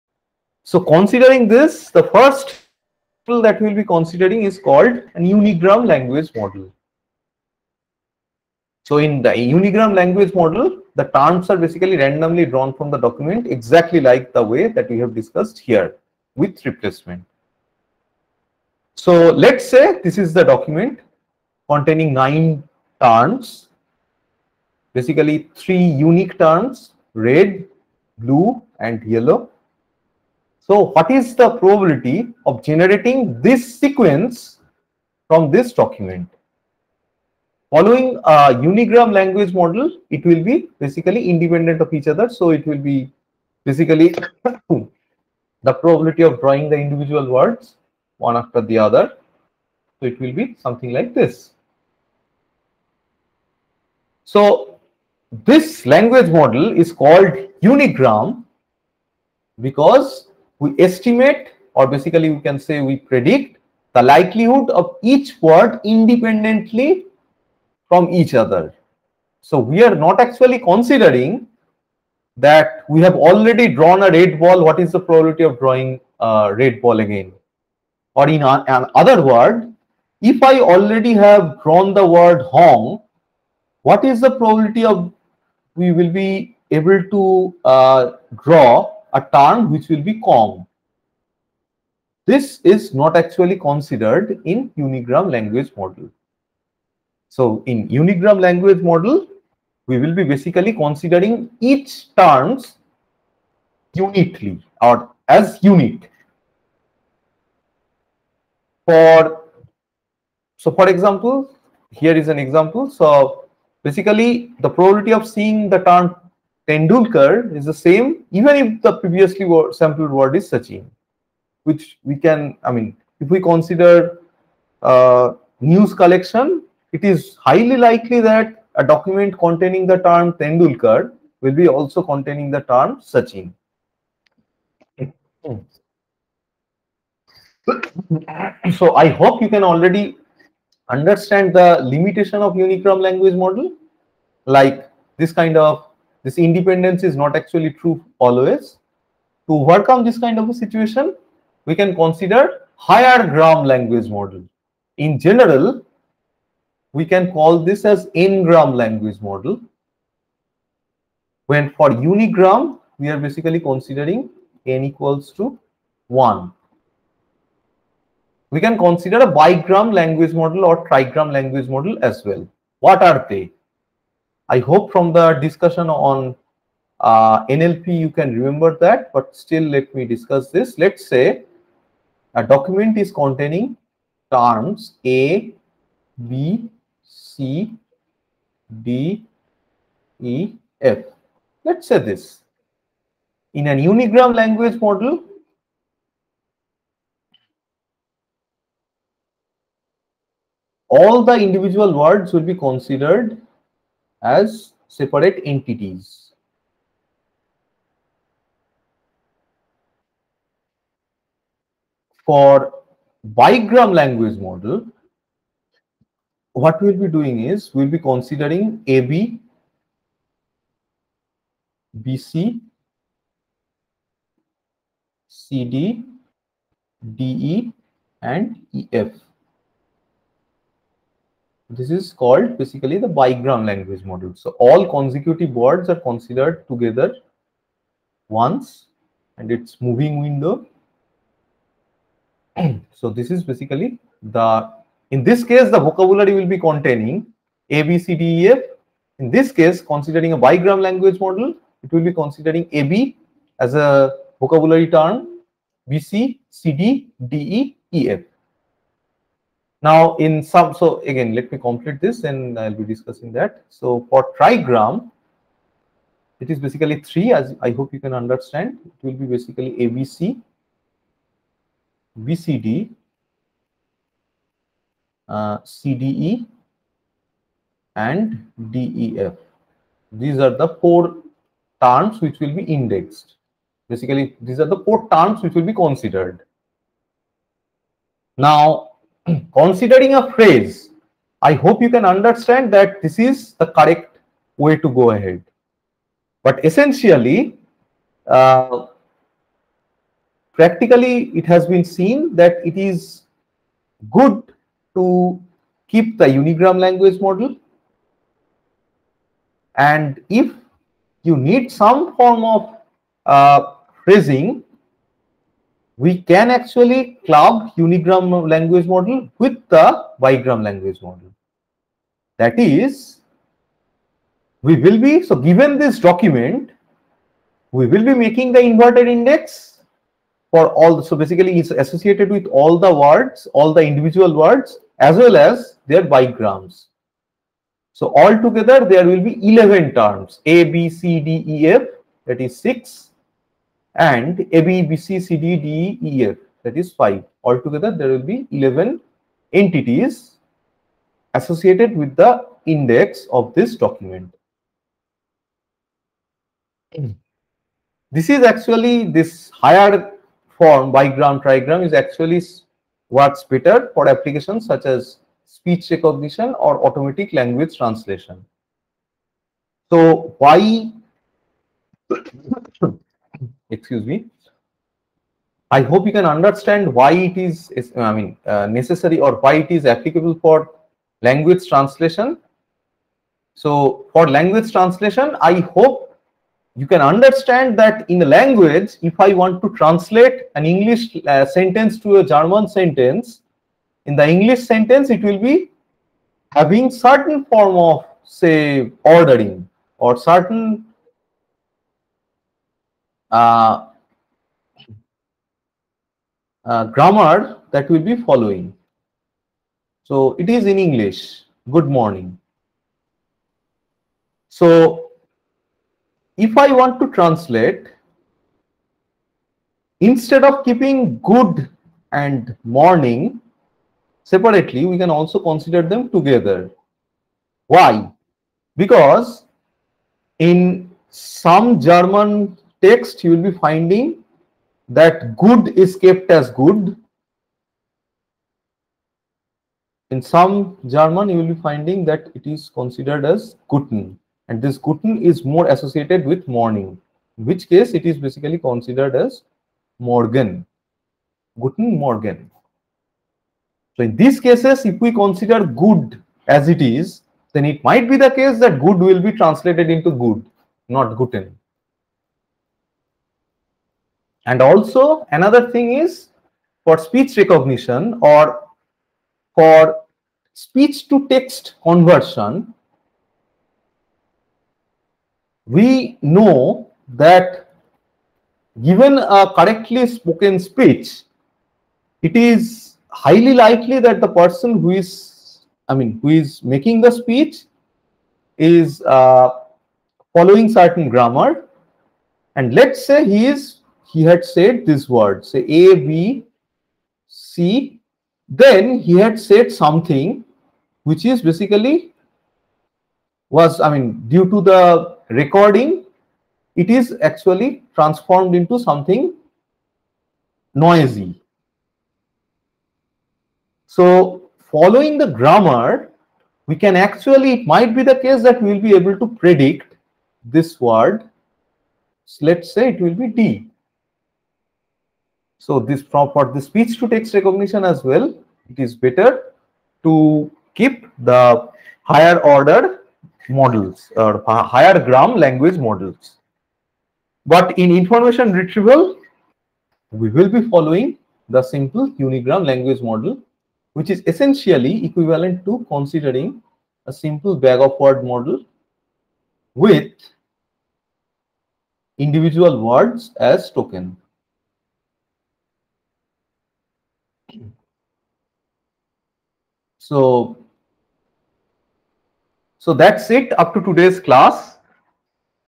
16.3. Language model for IR - Unigram language model
This is a course on Information Retrieval and Web Search offered to the students of IISER Kolkata in 2022.

The unigram language model is presented in the lecture with the use-case of document retrieval in mind.